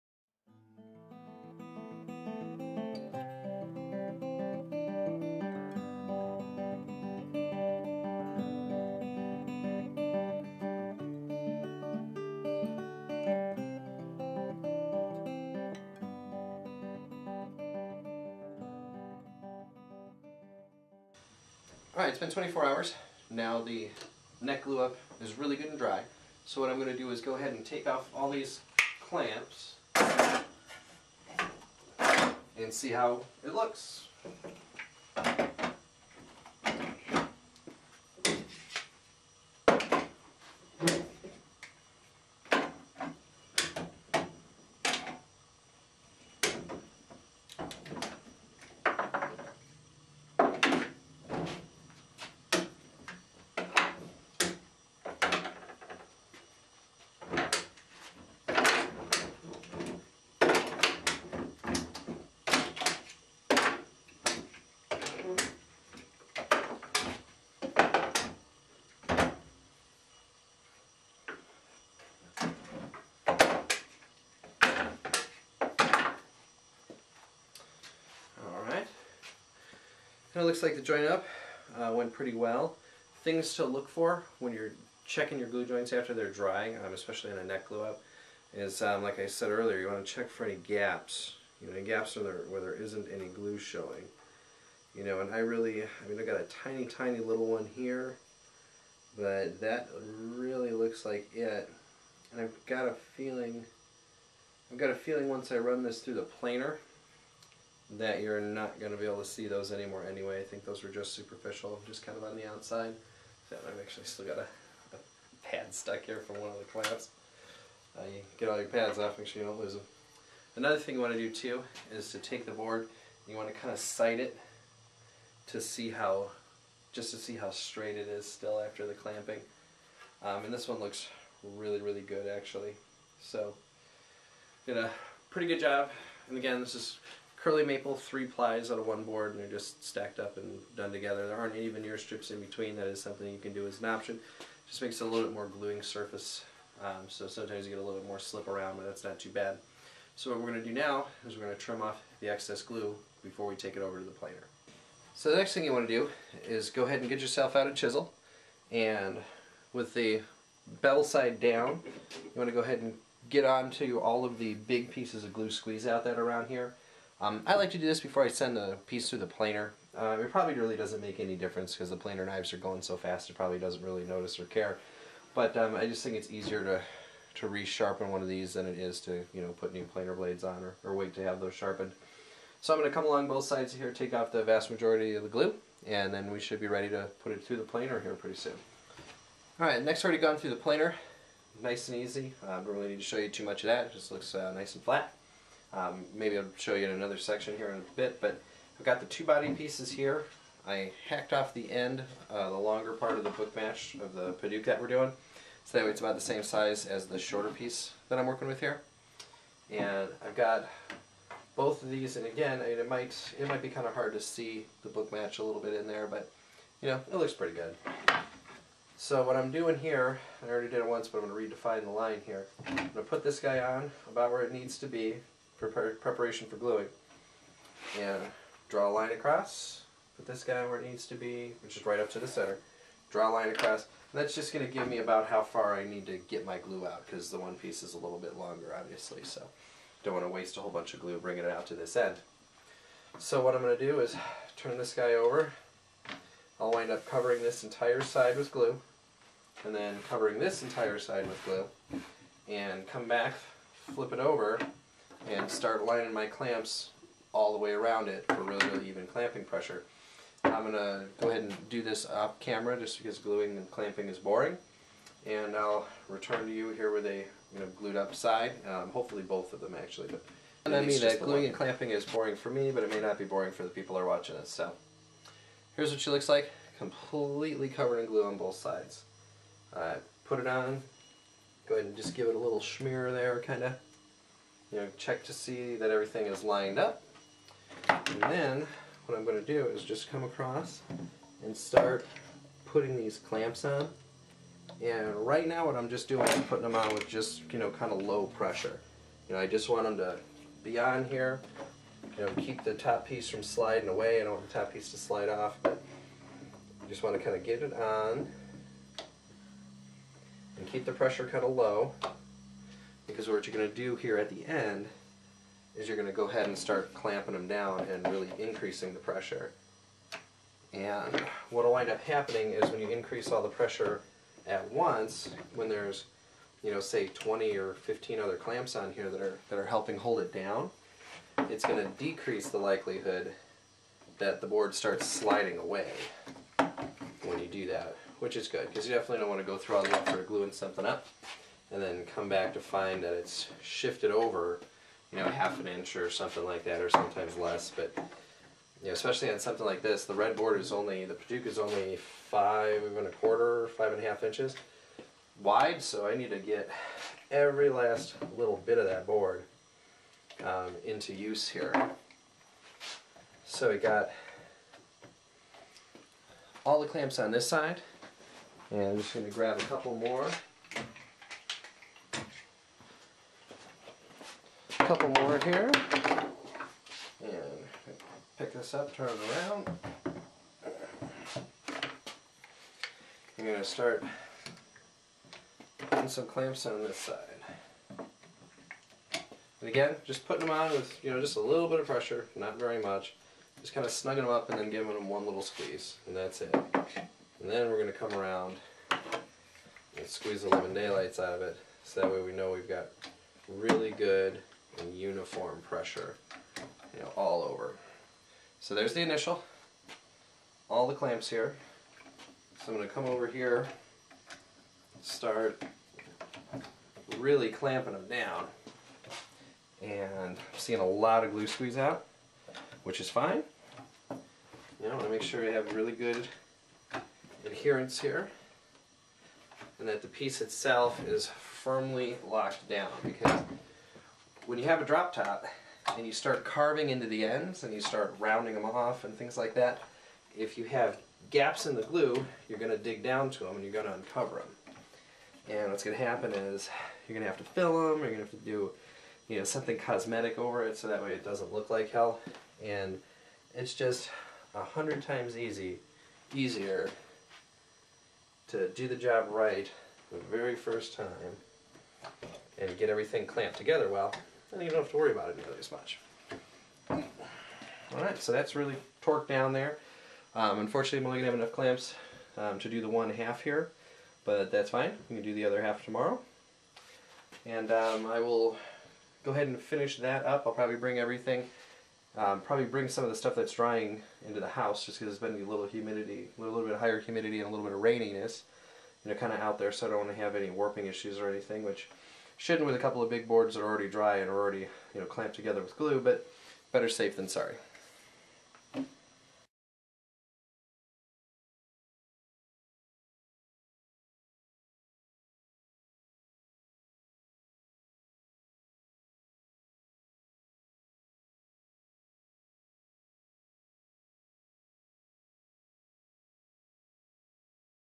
Electric Bass Project Part III: Glue-Up
This video covers the gluing of the Padauk drop top to the mahogany body blank, and how to get good results from the glue up.  It also shows the neck being removed form the clamps, and how to remove excess glue before putting the piece through the planer.